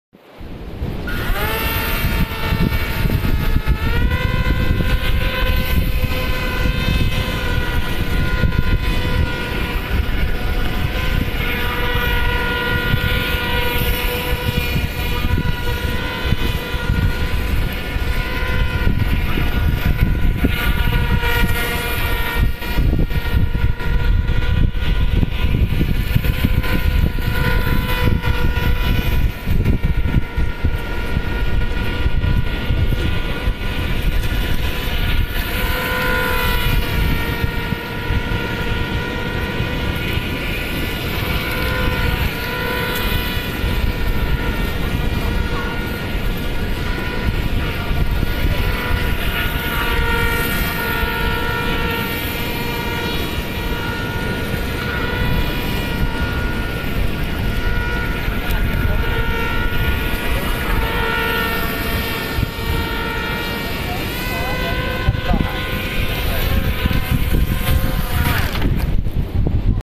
2021年1月19日
新做雙馬達小船,下水飆船